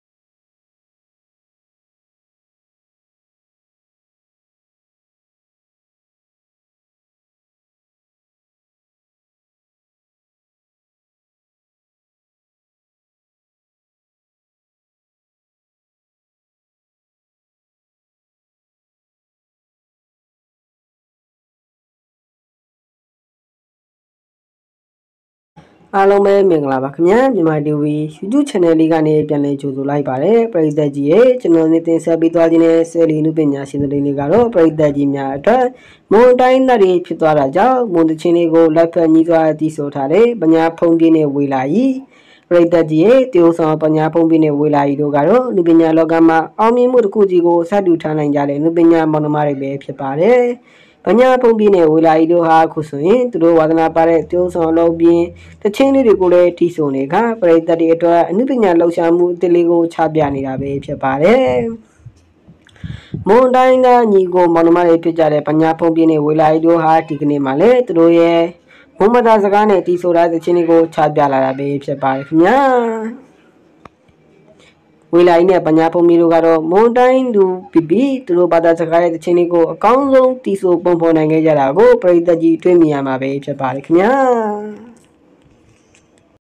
မွန်သီချင်းလေးကို လိုက်ဖက်ညီစွာသီဆိုထားကြတဲ့ဗညားဘုန်းပြည့်နဲ့ဝေလာရီcelebrity
မွန်တိုင်းရင်းသားတွေဖြစ်တာကြောင့် မွန်သီချင်းလေးကို လိုက်ဖက်ညီစွာသီဆိုထားကြတဲ့ ဗညားဘုန်းပြည့်နဲ့ ဝေလာရီတို့ရဲ့ဗီဒီယို

သီချင်းလေးက နားထောင်လို့ကောင်းလိုက်တာ
perfect